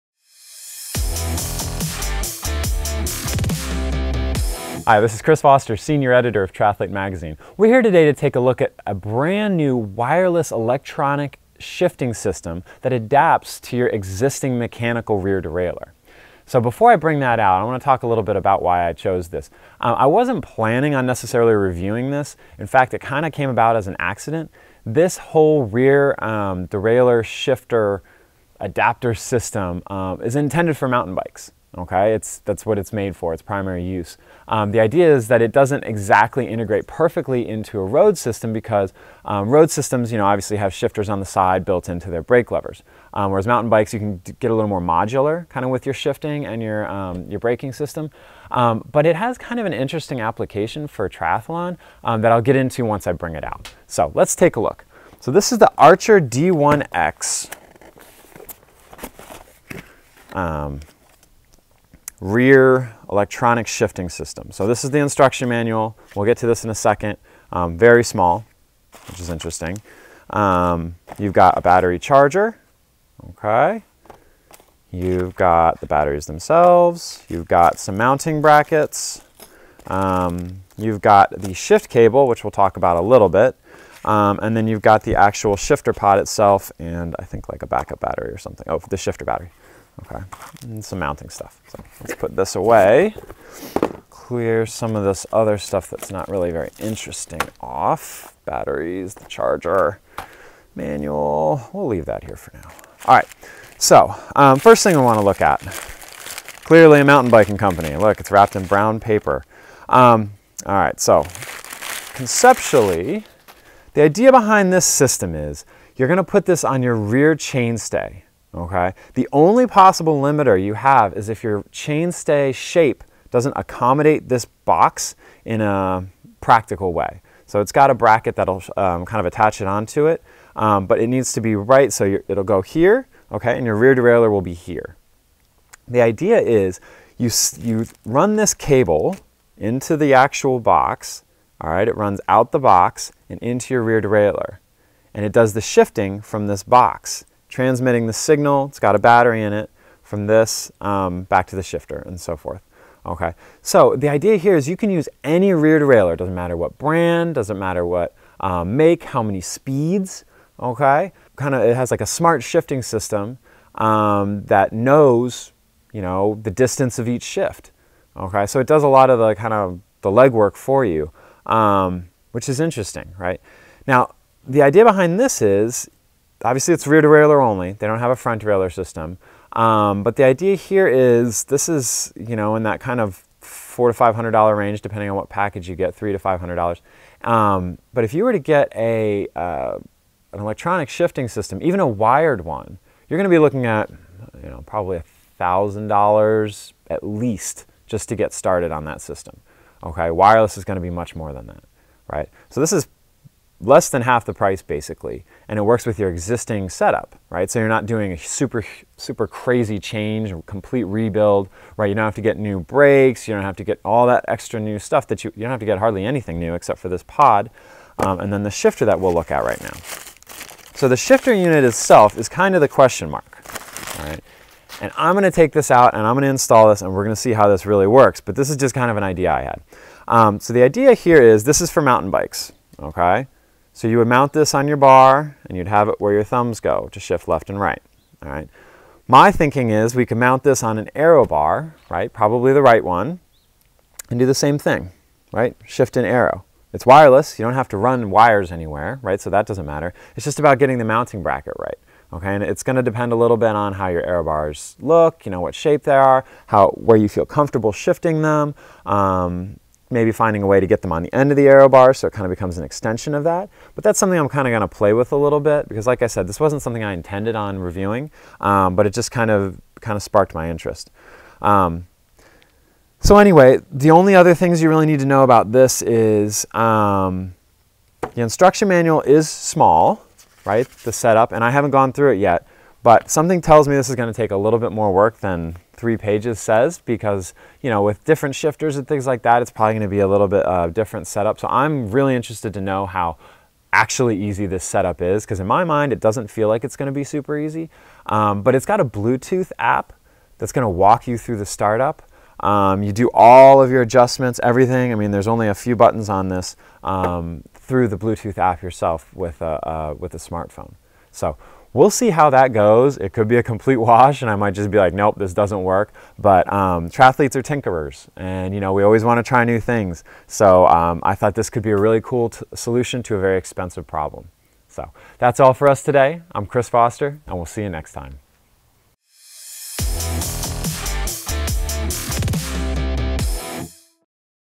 Unboxed: Archer D1x Trail Wireless Shifting
Senior Editor Chris Foster takes a quick look at an add-on shifting system that adapts existing mechanical rear derailleurs to become wireless. Though the device is designed for off-road use, could it be used for tri? We'll find out.